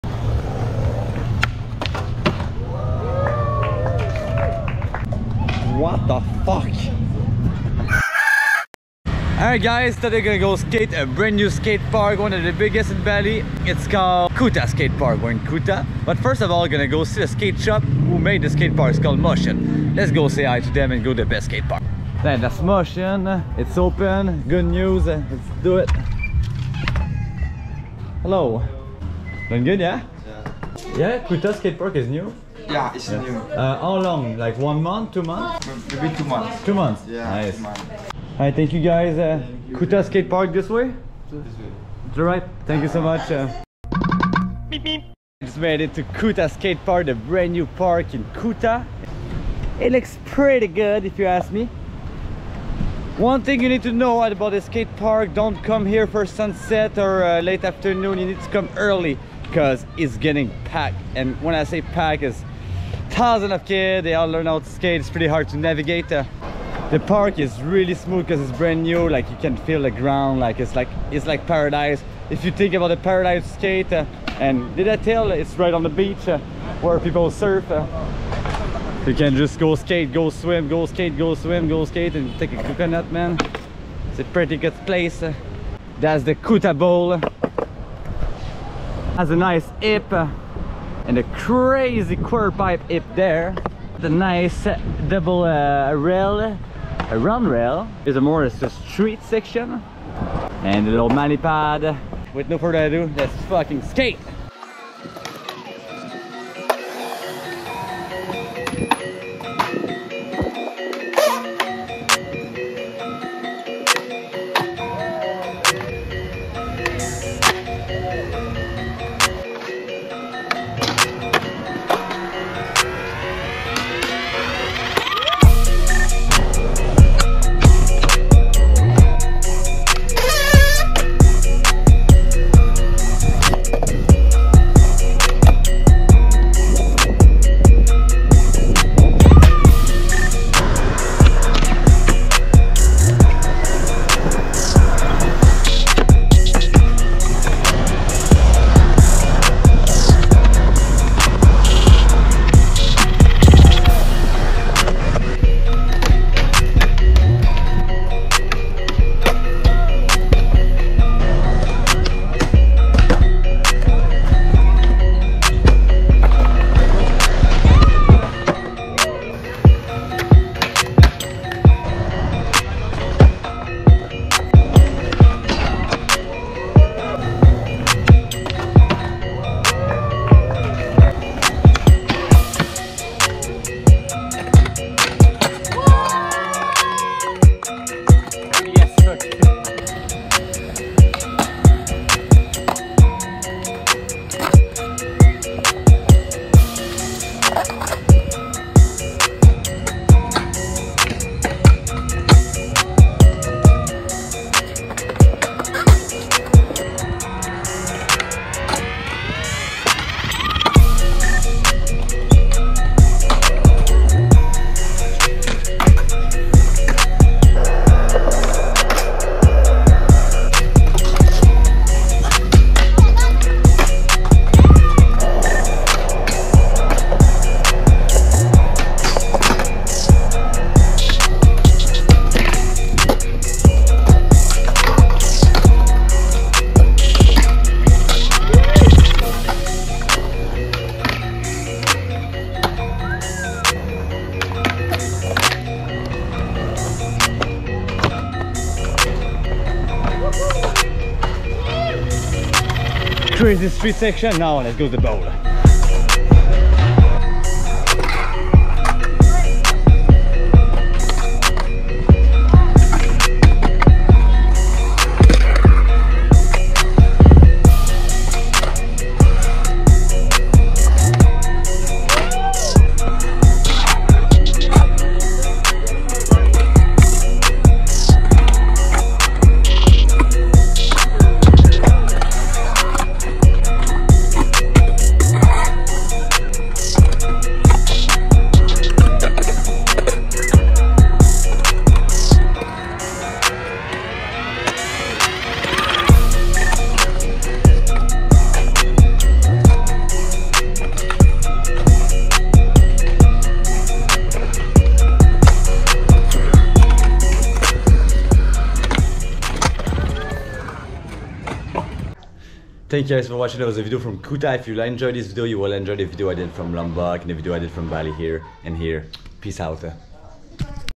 THE NEW KUTA SKATEPARK IN BALI IS INSANE 🇮🇩🛹
The new kuta skatepark in bali is what dream are made off . Built by the best skatepark company in Indonesia motion , the skatepark has everything a skater want . A perfect bowl and great street course . Right on the beach perfect to enjoy a sunset after .
Much love - Mike